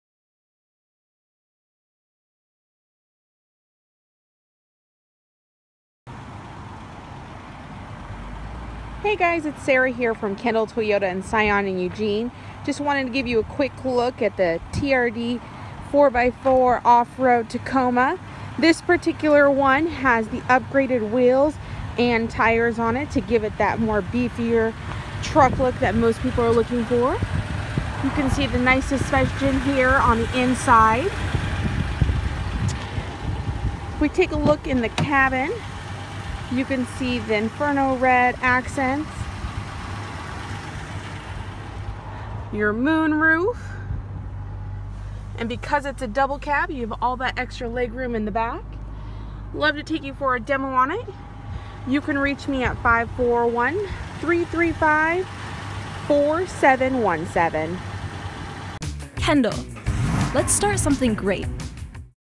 2016 Toyota TRD OFFRoad Tacoma with Wheel and tire package
Check out our 2016 Toyota Tacoma TRD OFF Road  with after market wheel and tire package.
373 Goodpasture Island Rd
Eugene, Oregon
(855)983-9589